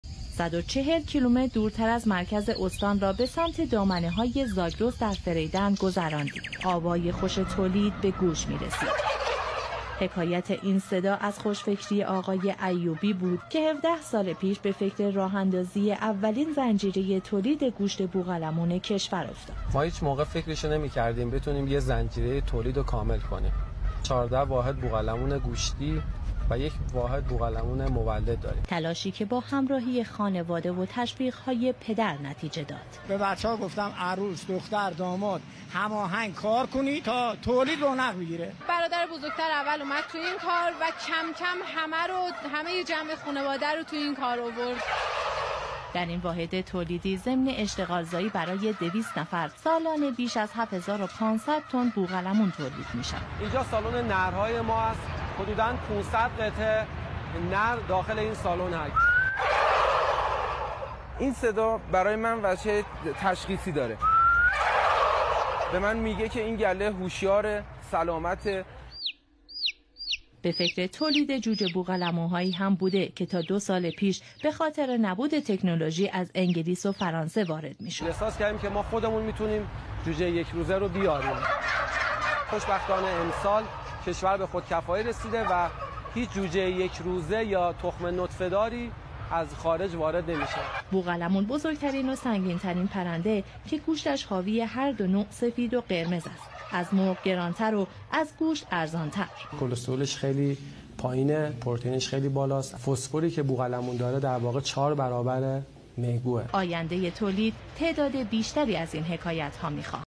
Iran Turkey birds farming, Nanadegan village, Fereydan county پرورش بوقلمون روستاي ننادگان فريدن
May 6, 2019 (Persian calendar 1398/2/16)

Nanadegan village (روستاي ننادگان) Geo coordinate
32°50′54″N, 50°21′02″E

Iran Turkey birds farming, Nanadegan village, Fereydan county
پرورش بوقلمون روستاي ننادگان شهرستان فريدن ايران